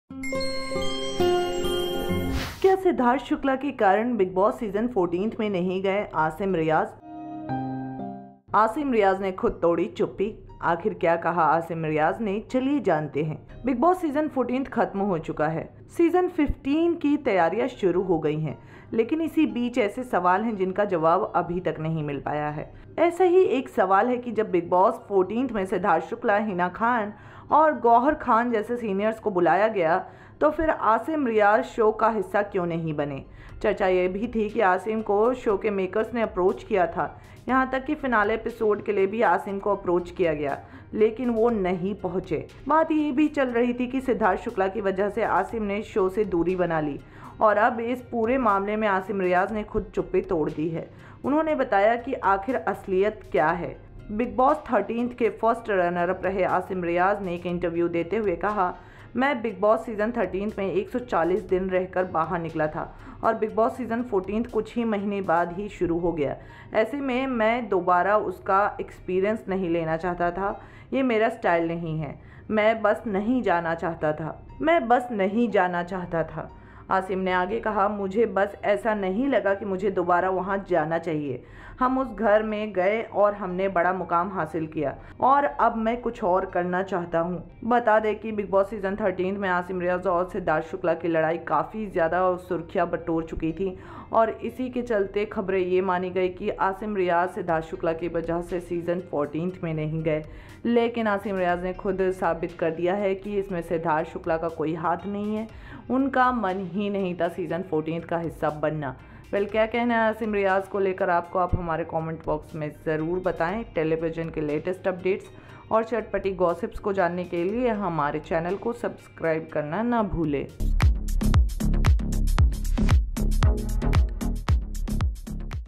Bigg Boss 14: Asim Riaz Reveals Why He Rejected An Offer To Be Part Of Salman Khan's Show
Asim Riaz has revealed that he was asked to return to Bigg Boss 14, but he rejected the offer. Here's why.